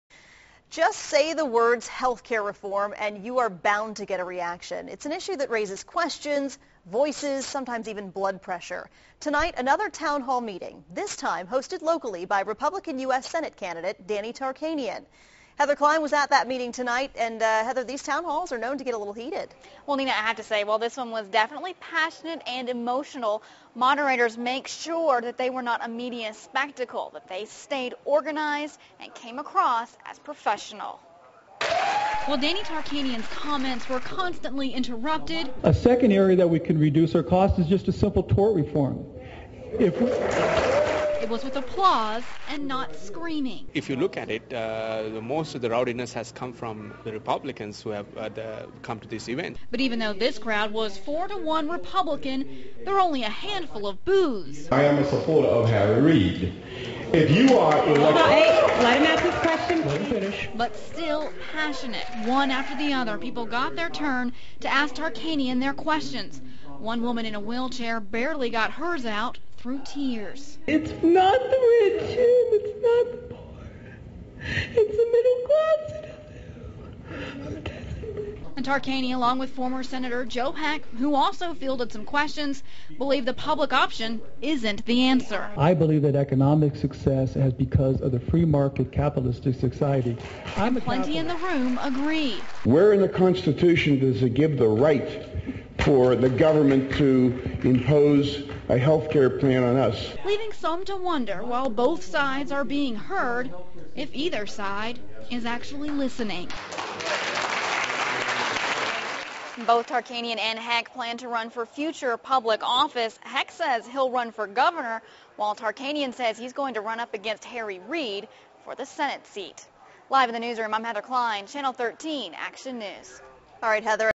Danny Tarkanian, Harry Reid's opponent, hosts town hall meeting in Las Vegas
Danny Tarkanian, Republican challenger to Harry Reid, hosted a town hall in Las Vegas, NV.  He was the first person to announce that he'd host a town hall in Las Vegas.  He announced that the day he formally announced his candidacy on Newsradio 840 KXNT with Heather Kydd and Casey Hendrickson.  Another station heard this, and hosted their own town hall before Danny's.  It was a disaster.

Heather Kydd and Casey Hendrickson were asked to moderate the Tarkanian event, and there was no bad press.  The crowd was very well behaved, professional and still passionate ... on both sides.  It was an amazing event, and the only one hosted by a politician so far.

Harry Reid sent people down to the event ... not just supporters, but some of his people.  Which was ironic given Reid's allegation of astro turfers at town halls.  The Reid people were welcomed with open arms, behaved themselves, and asked tough questions.

Danny Tarkanian showed everyone how a town hall should be conducted, and he did it because he was asked to by voters.  That really says something about his character ... whether you agree with his politics or not.  Meanwhile, Reid is devoting 1 hour to a telephone town hall for 50,000 people ... laughable.